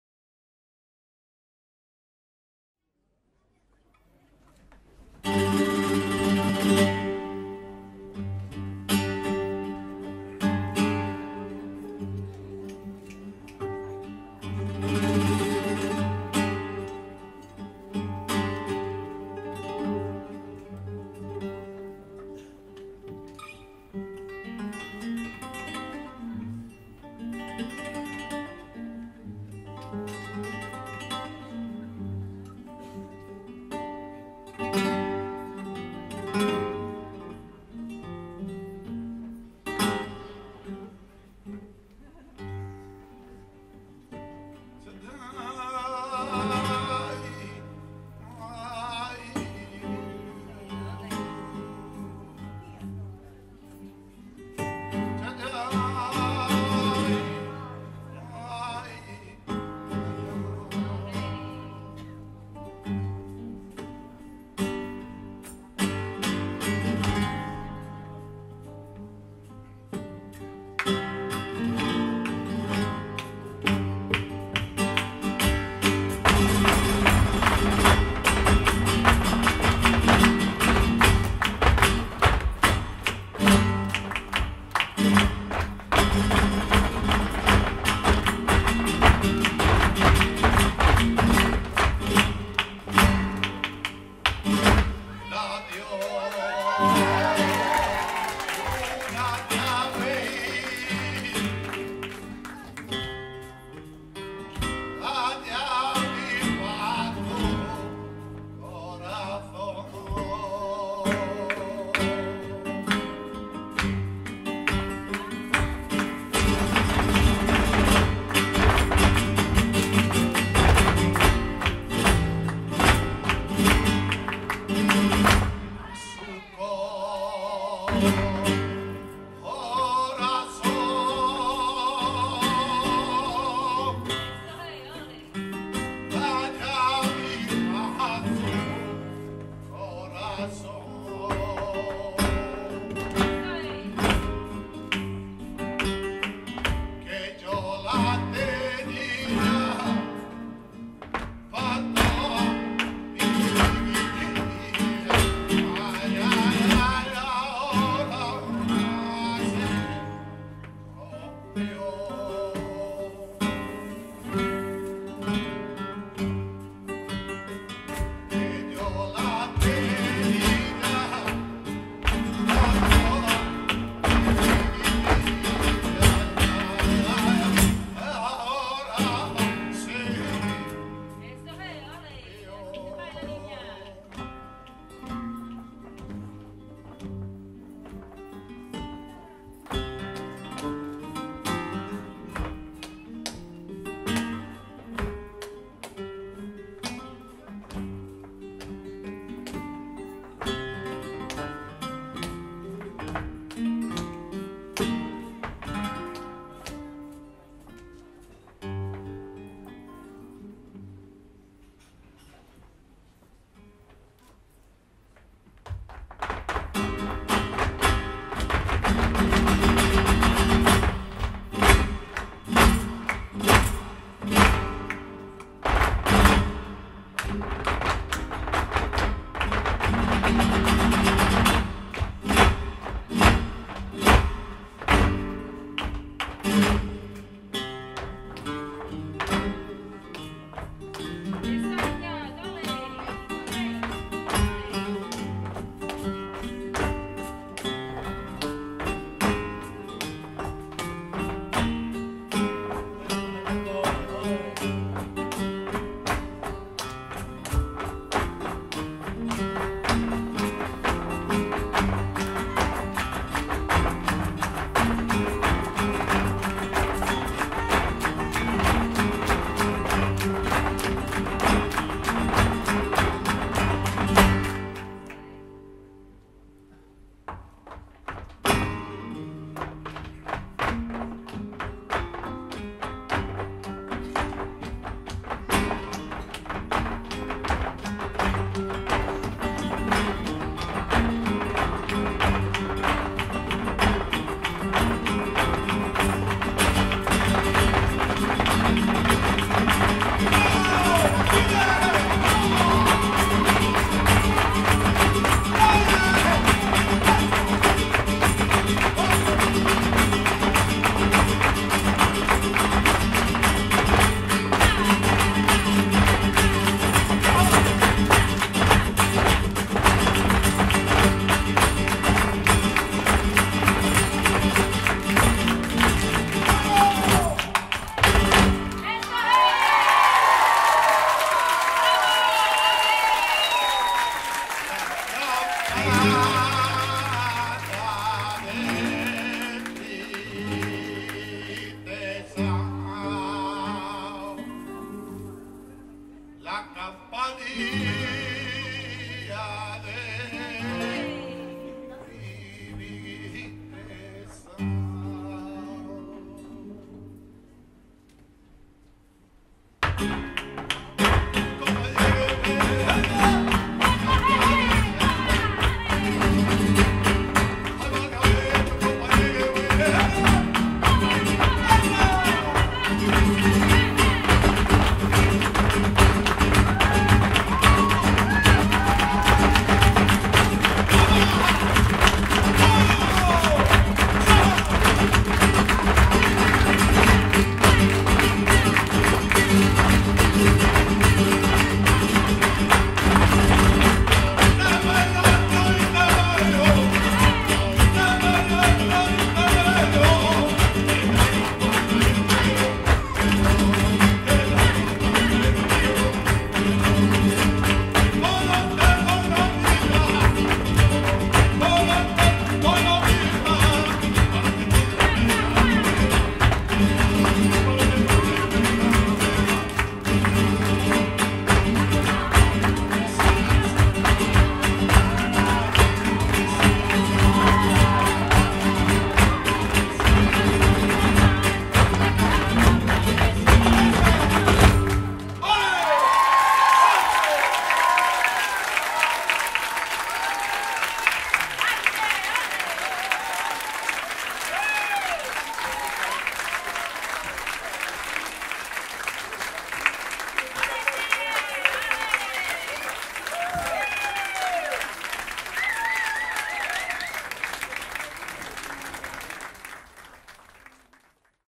Flamenco Avancerad vt 2013
Avanceradgruppen i Flamenco framför sitt uppvisningsnummer på World Dance Companys avslutningsshow för vårterminen 2013.
Filmat på Musikens hus i Göteborg.